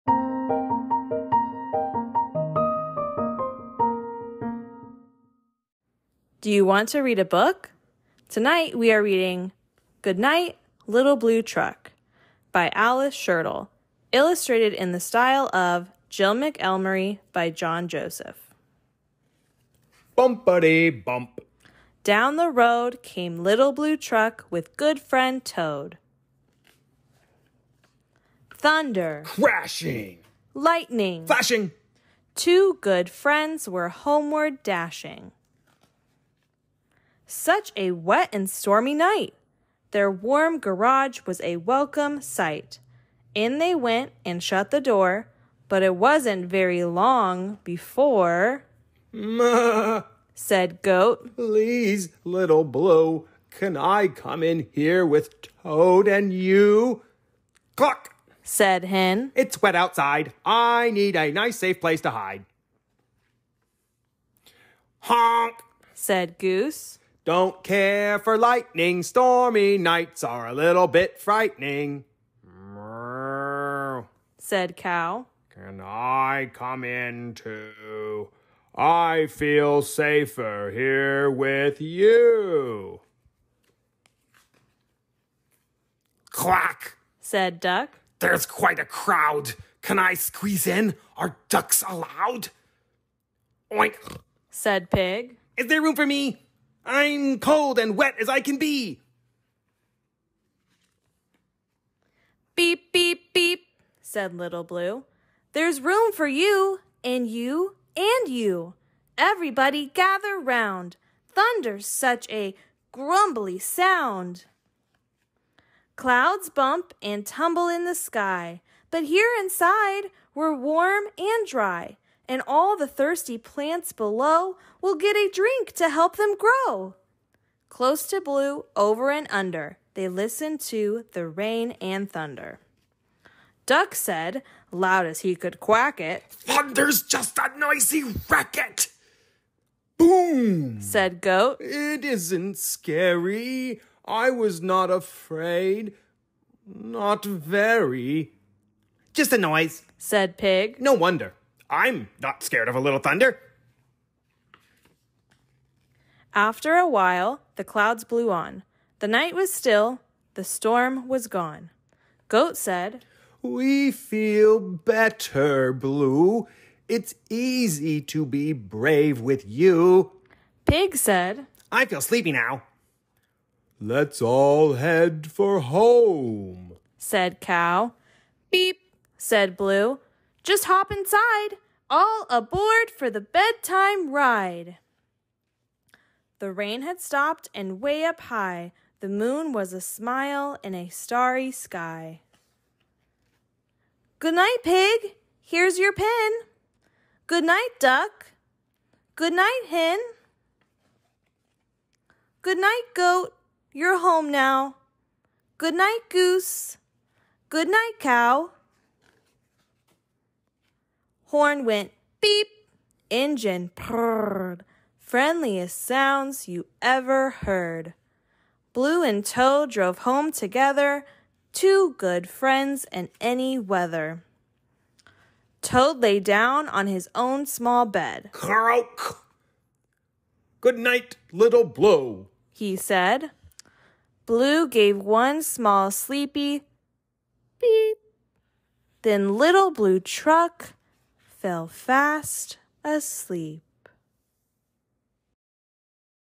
"Good Night, Little Blue Truck" Read Aloud - Ever After Stories For Kids
Listen to the story of Little Blue Truck as he and his friends prepare for bed. Written by Alice Schertle / Illustrated by Jill McElmurry.